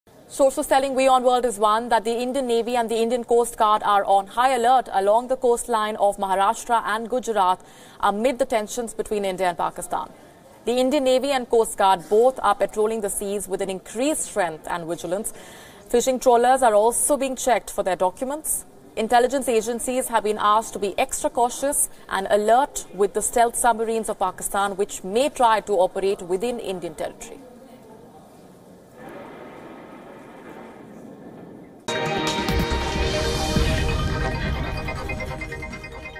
Sources: Indian Navy and Indian Coast Guard on high alert
The Indian Navy and Indian Coast Guard are on high alert on Wednesday along the coastline of Tamil Nadu amid escalation of tension between India and Pakistan after Indian Air Force carried out strikes on a Jaish-e-Mohammad (JeM) terror camp in Pakistan’s Balakot on Tuesday.